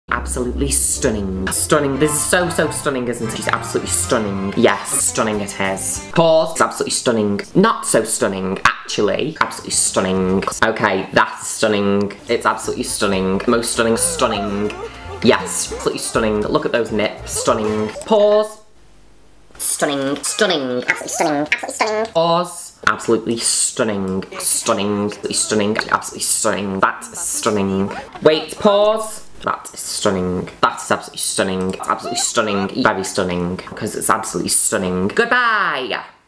Juno Birch - Stunning
How many times does Juno Birch say STUNNING in a single video???
I absolutely adore her! She's absolutely STUNNING!!

Unemployed digital content creator. I make my own MEMES, EDITS and VIDEOS. Like my edits?